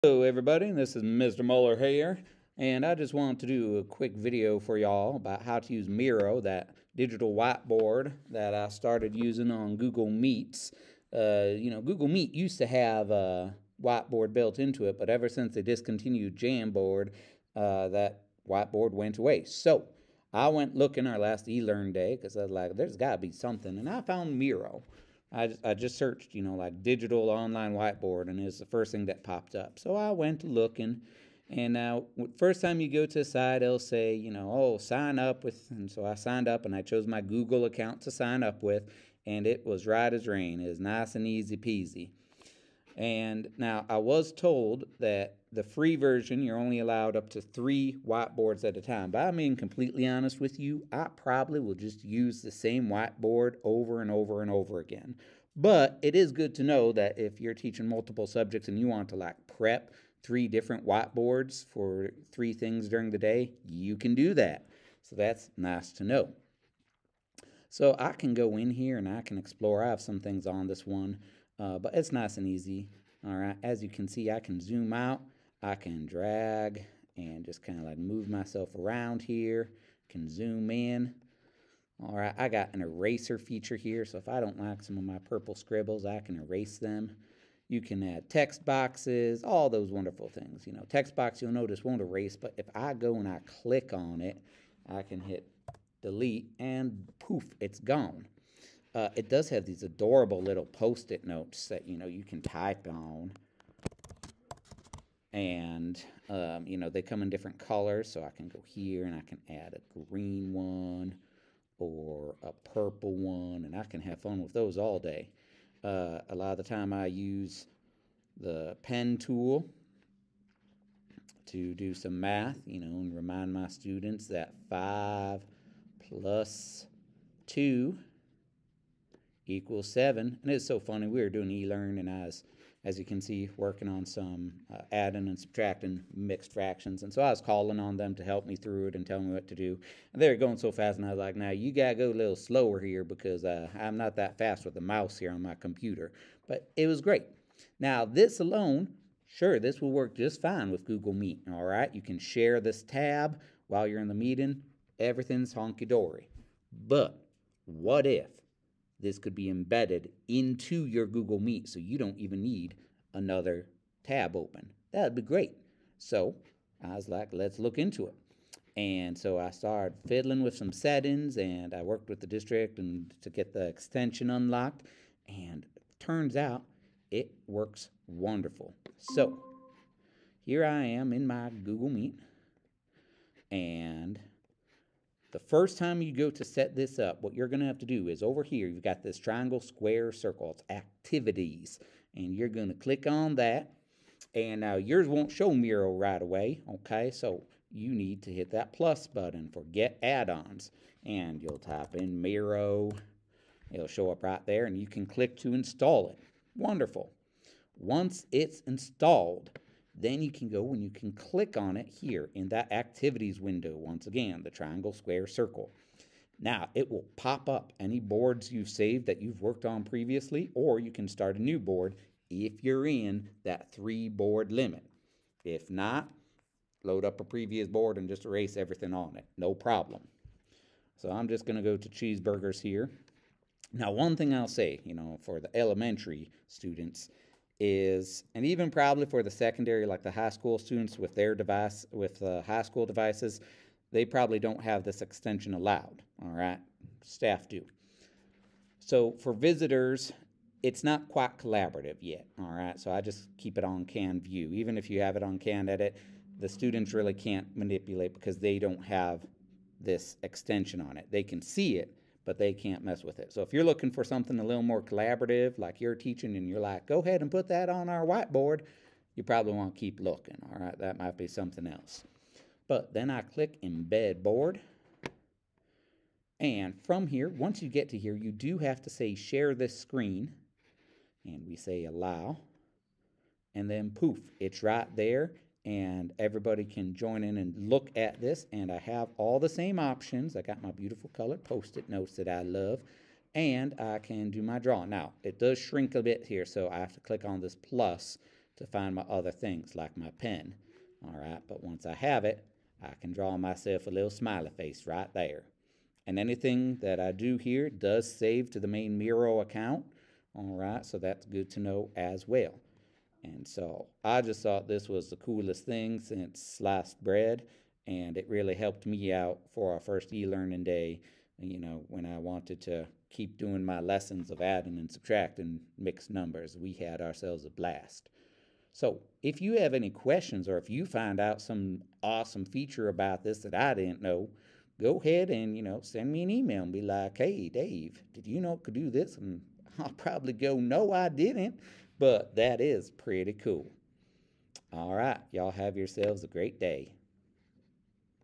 Using Miro Whiteboard with Google Meets
Miro is a wonderful way to add whiteboards to your Google Meet!